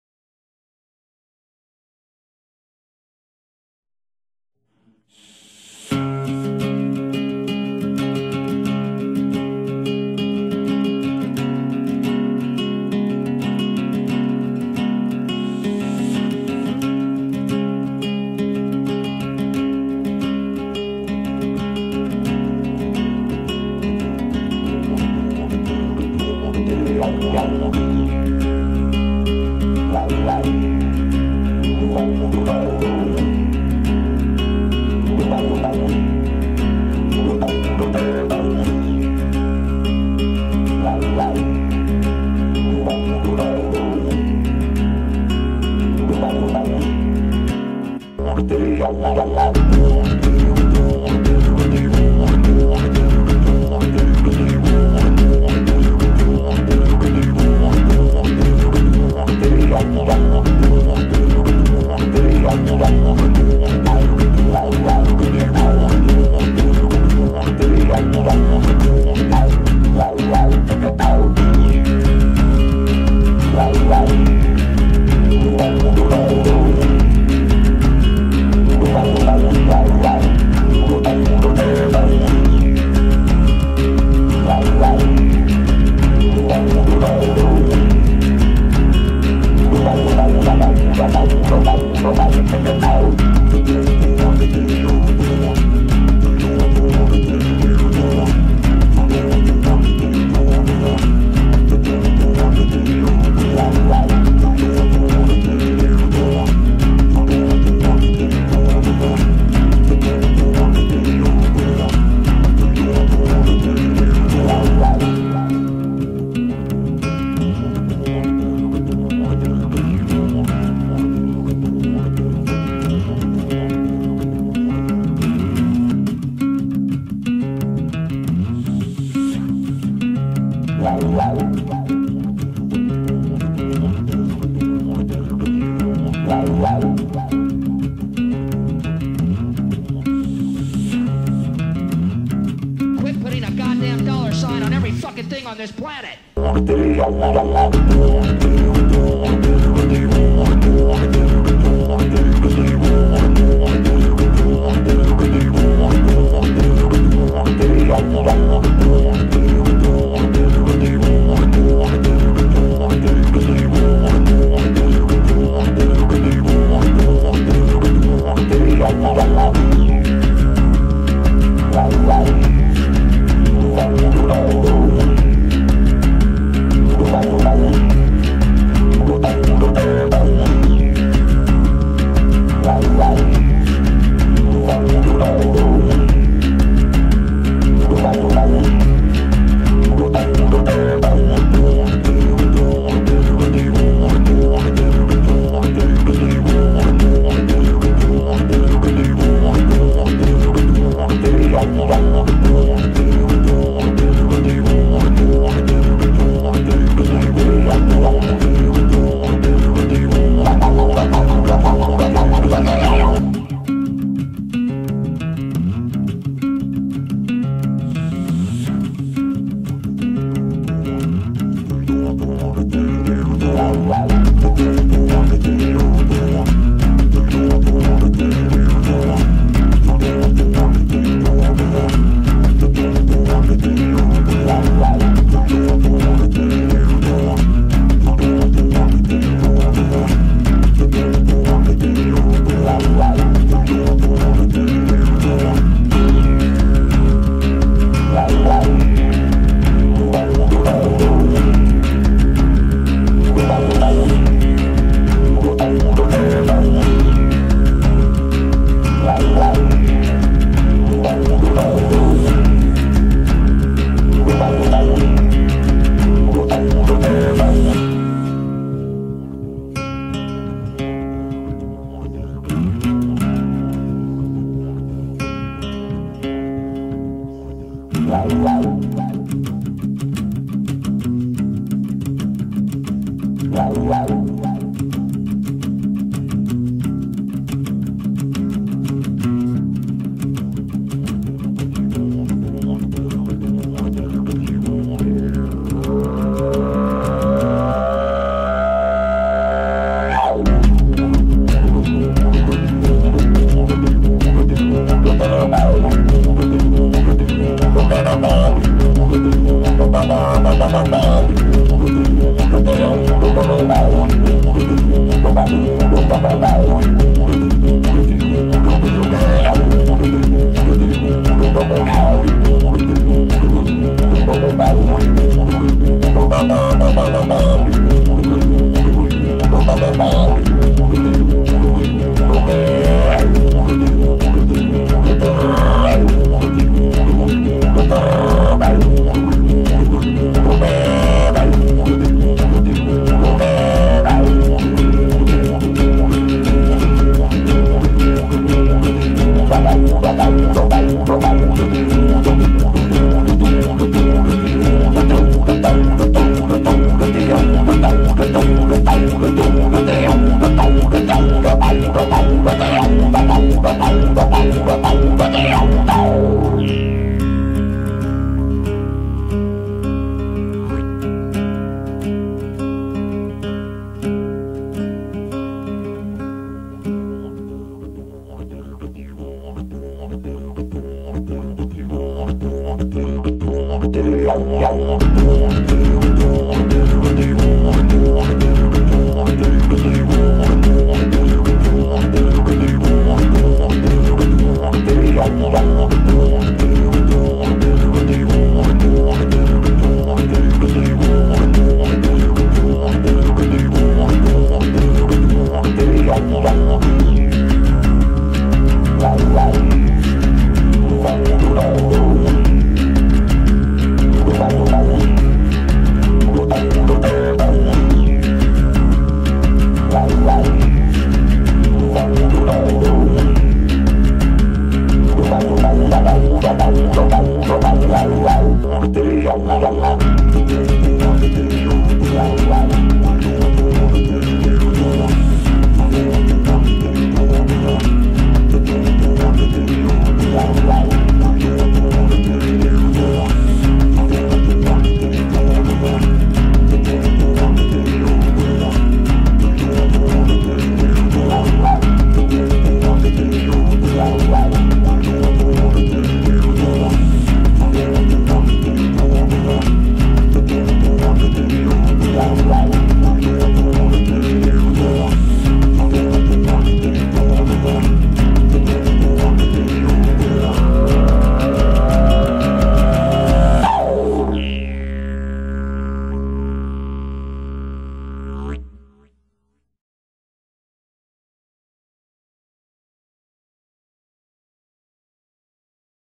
didgeridoo gitara akustyczna & bas
skomponował,grał i zmixował m.owczarek.w rurke dmuchał i clip zmontował m.jamakam.materiał archiwalny wykorzystany w tym filmie pochodzi z mojej kolekcji utrwalony dzieki pomocy serdecznych przyjaciół.filmy powstały w roku2005,2007 w miejscach mocy na terenie leżajska,oraz z ulicy w galway(2009).pozdrowienie dla całej ekipy która brała w tym udział.